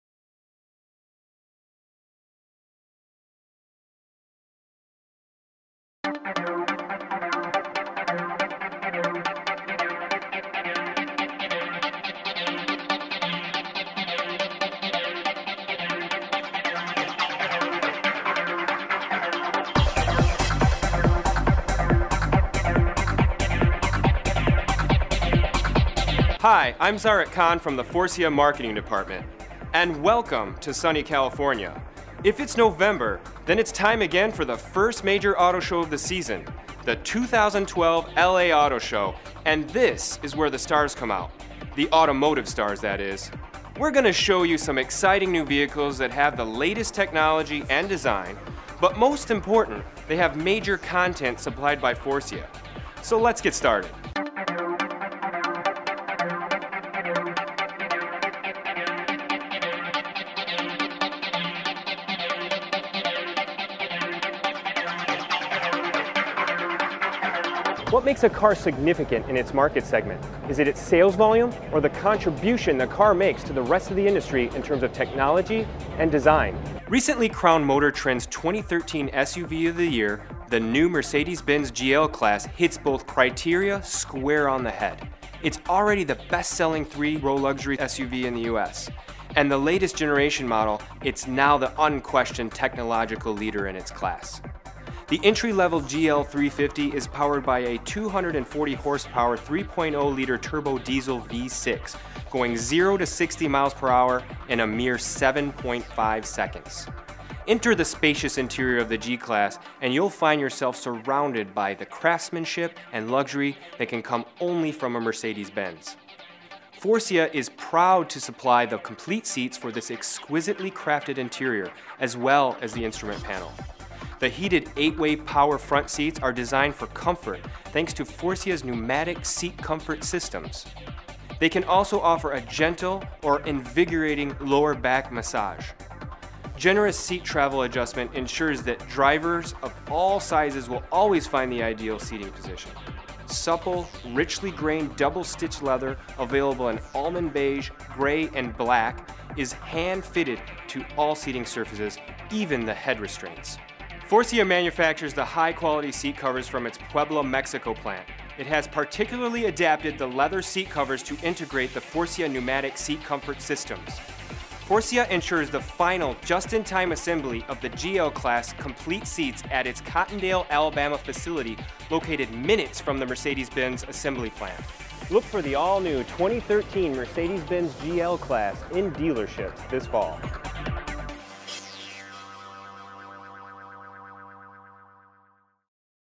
Faurecia at the LA Auto Show with the Merecedes-Benz GL-Class
This year at the LA Auto Show, Faurecia took a closer look at the Mercedes GL-Class, which is equipped with seating and instrument panels from Faurecia.